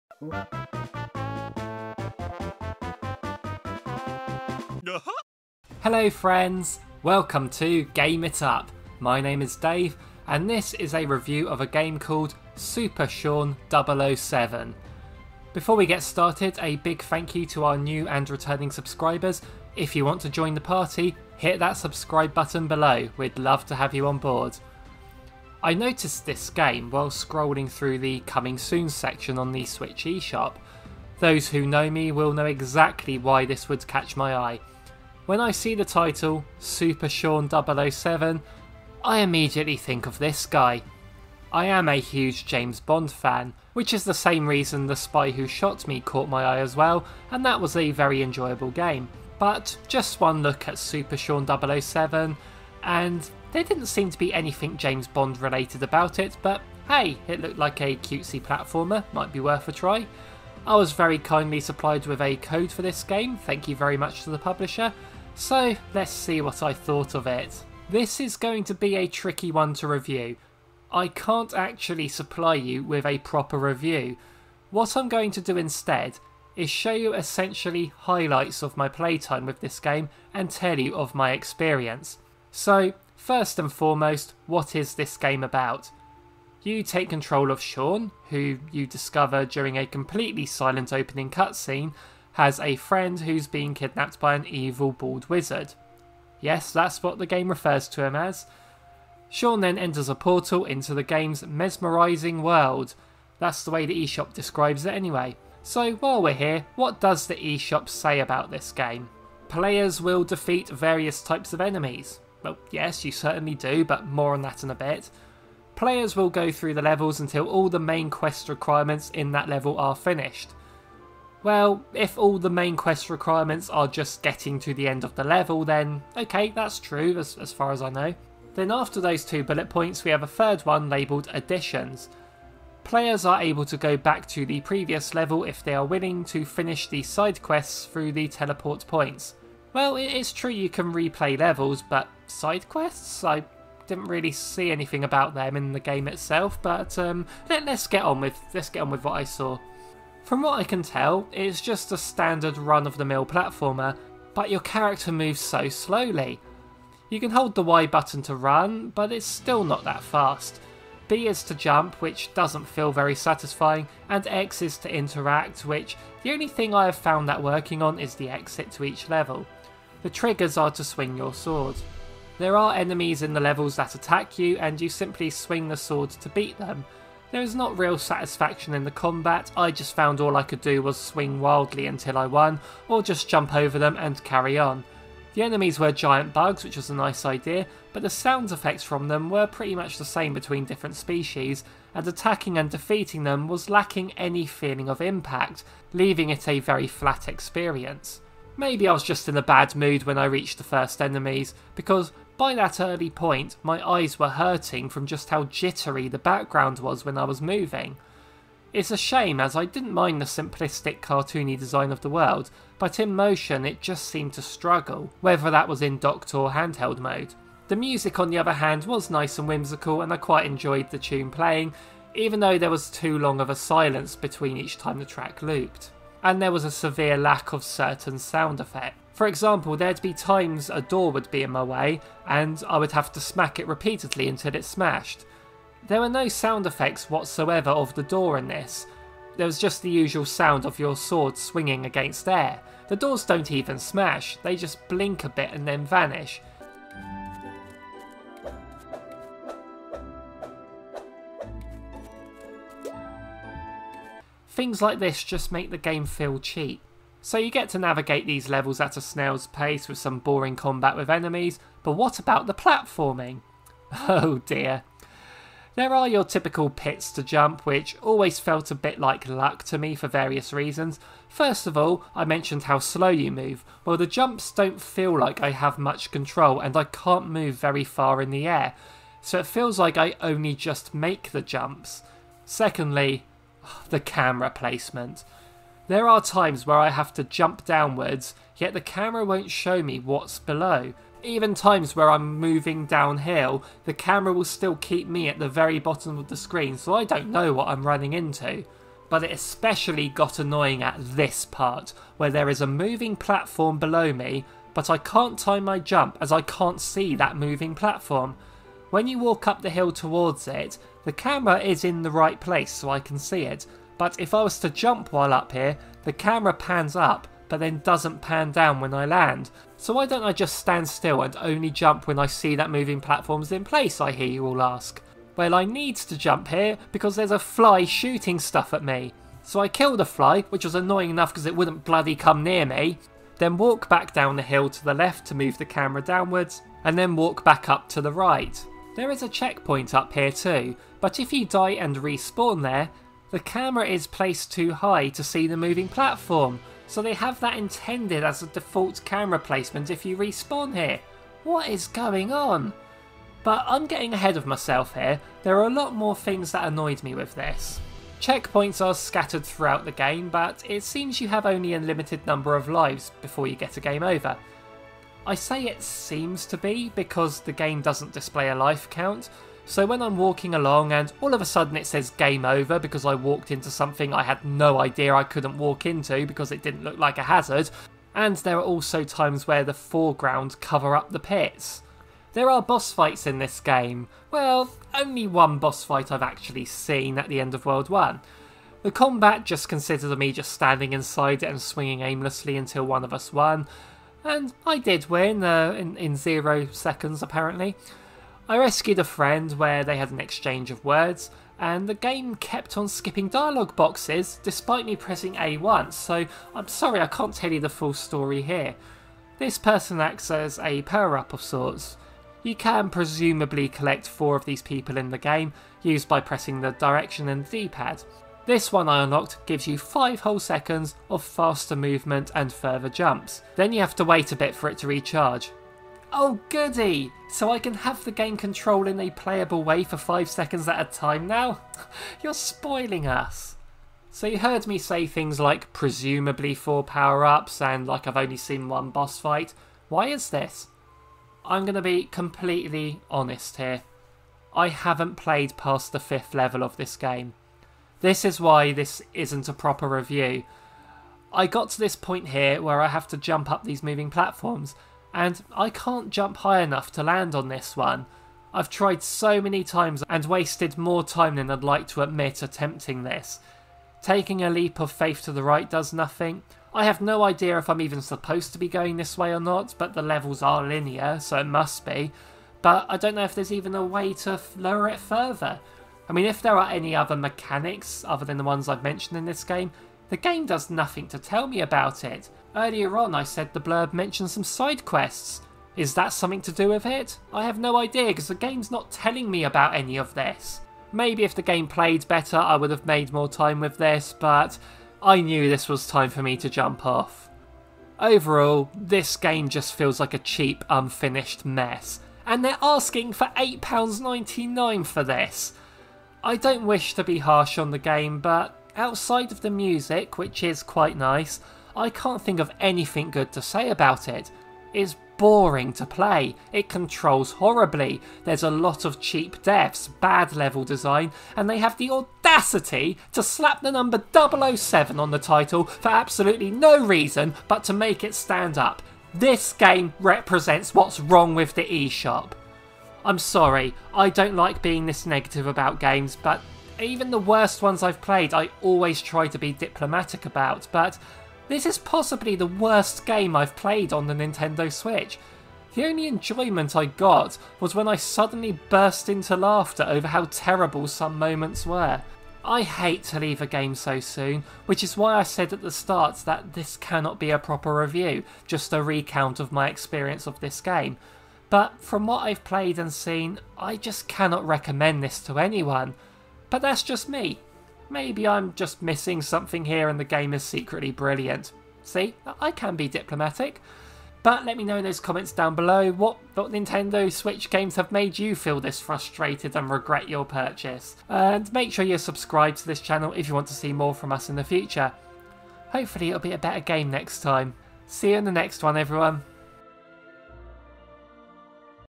Super Sean 007 Review (Nintendo Switch)- Not Super and Not 007
Super Sean 007 is a 2D Platformer set in a mesmerizing world... brace yourselves guys.. I don't often get so negative in reviews but there was no helping this one.